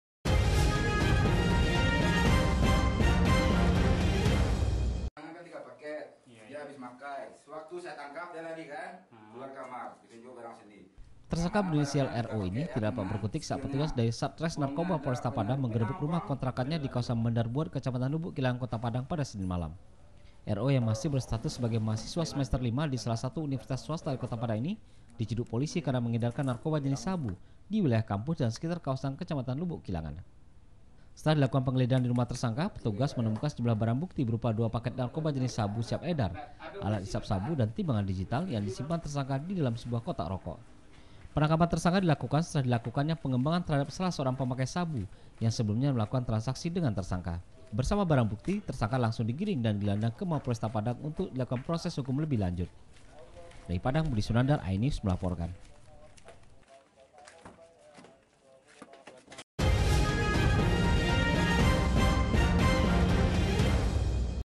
Mahasiswa Bandar Sabu Digerebek
Seorang mahasiswa disalah satu perguruan tinggi swasta di Kota Padang, ditangkap Tim Satres Narkoba Polresta Padang disebuah rumah kontrakan dikawasan Bandar Buat, Kecamatan Lubuk Kilangan.